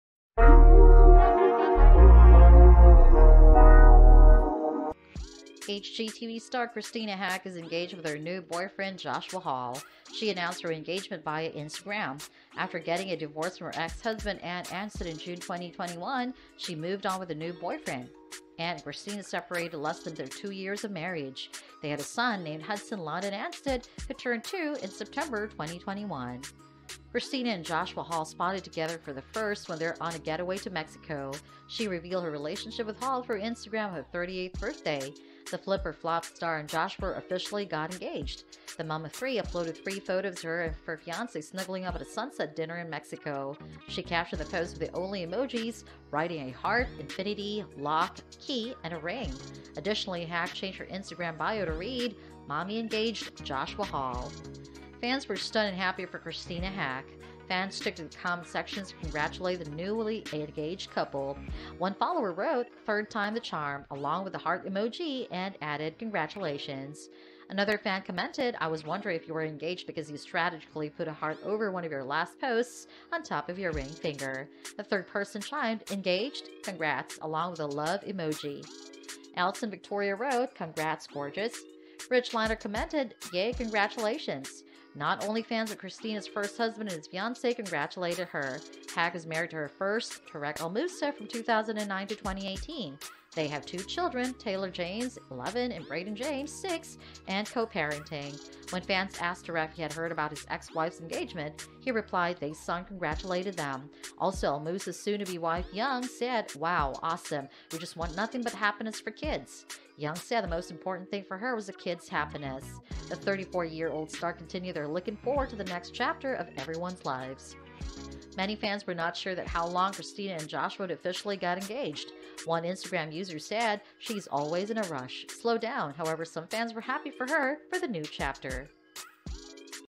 Tarek El Moussa and His Fiance Heather Rae Young React to Christina Haack's Engagement
Christina on the Coast star Christina Haack got engaged to her new boyfriend, Joshua Hall, after months of dating. Christina's first husband, Tarek El Moussa, and his fiance reacted to Christina's engagement and congratulated the couple.